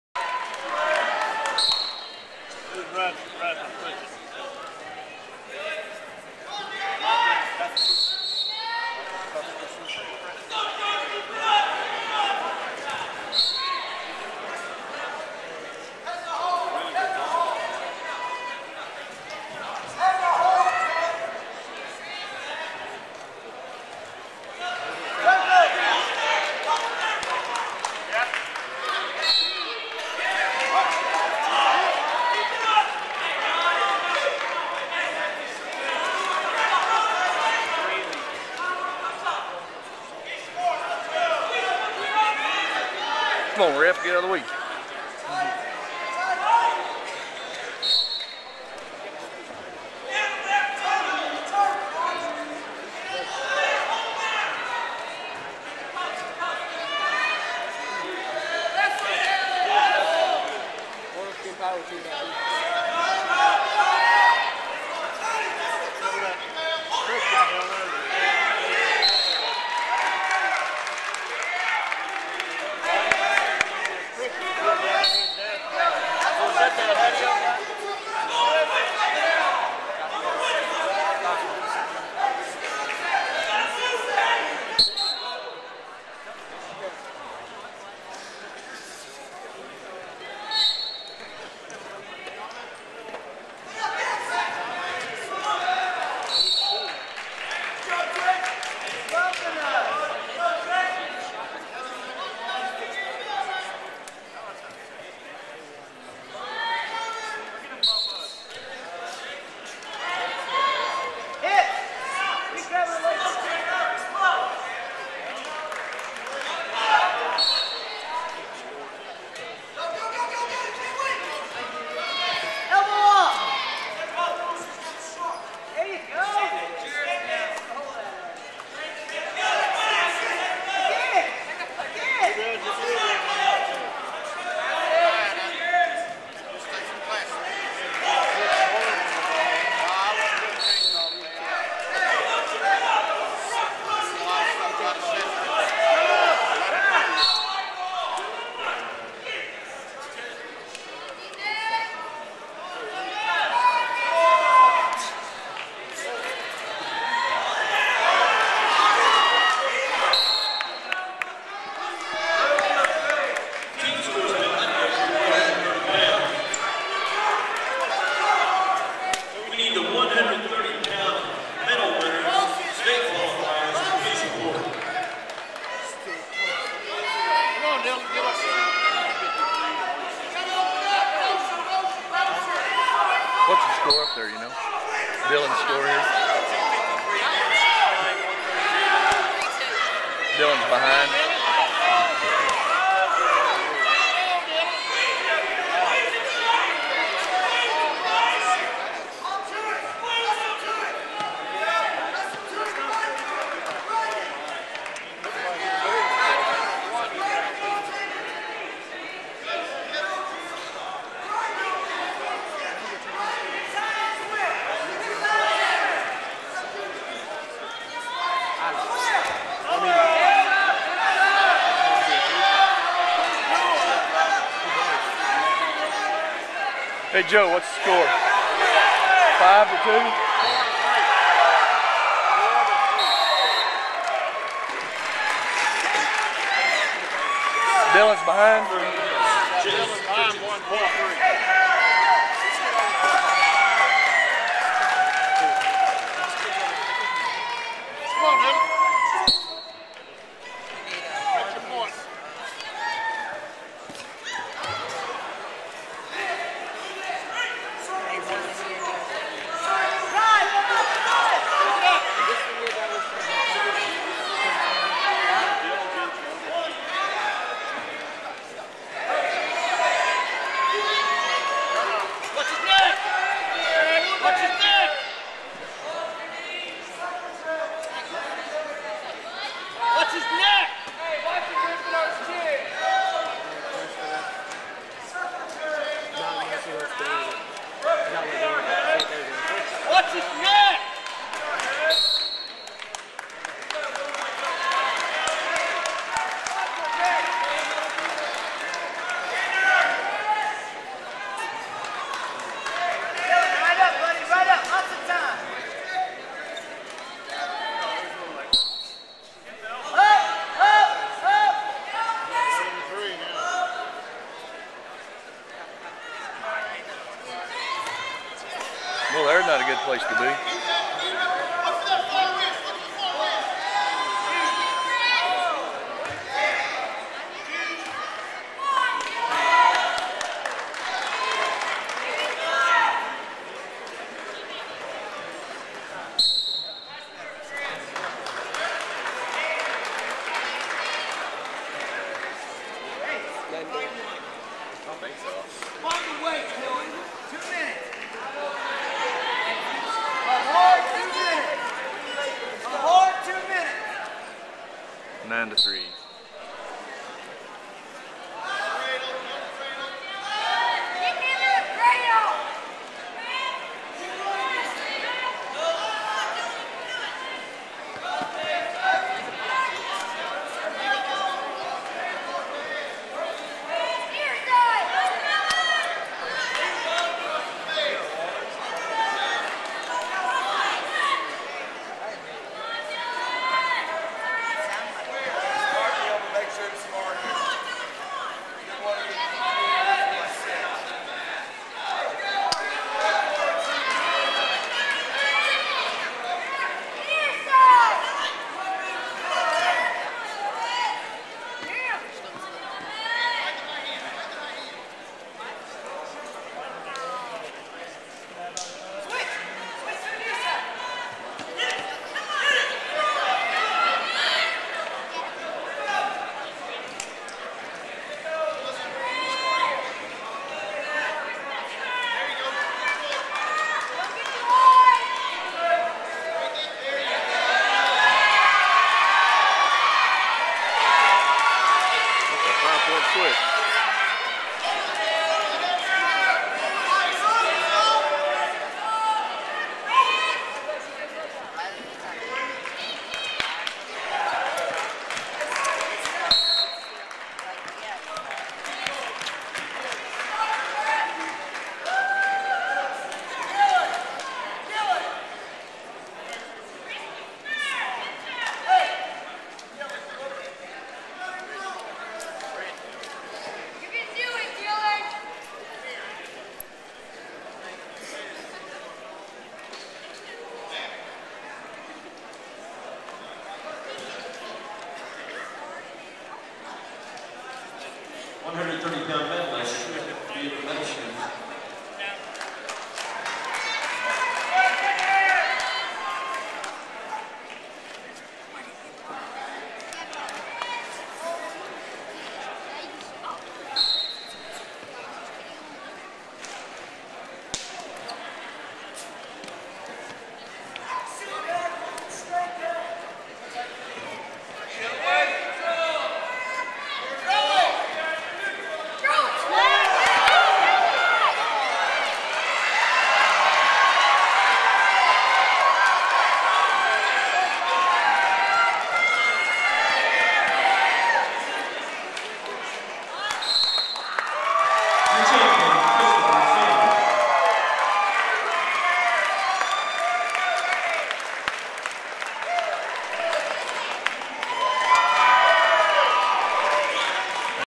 Chris Sanders (Rolla) vs Dylan May (Ozark)
Recorded on February 13, 2010 using a Flip Video camcorder.  Finals of the 135 lb weight division - Class 3 District 3 Tourny in Ozark Missouri Feb 12-13, 2010